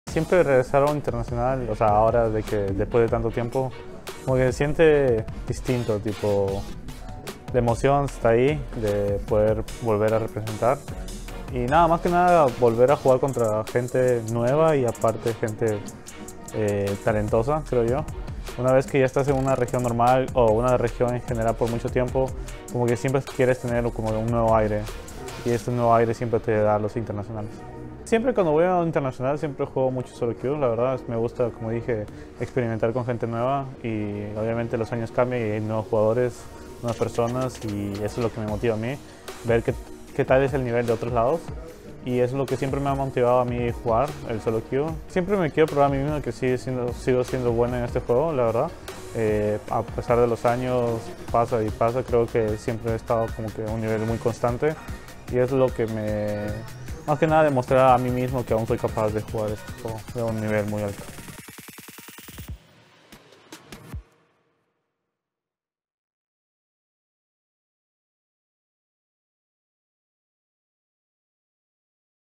Entrevista con Oddie: Un nuevo desafío internacional | Previa MSI2023 | League of Legends
El jungla de R7 desafiará a nuevos y viejos conocidos en el MSI 2023. Hablamos con él para saber sus expectativas de cara al debut de Movistar R7 este 3 de mayo.

Sigue la transmisión y consulta los horarios en https//lolesports.com